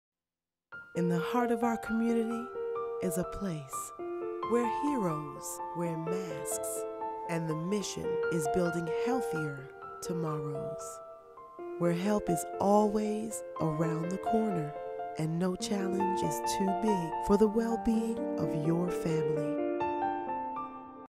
TV Commercial Ladie Most
TV Commercial for a hospital in Birmingham, AL, voice-over by Ladie Most.